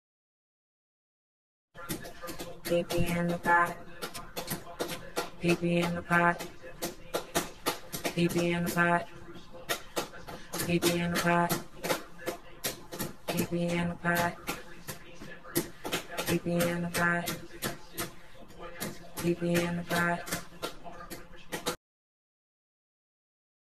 Shawn Donald Morris ( Potty training & Drumsticks)
Potty training working in his favor & mommy favor...lol 2birds 1stone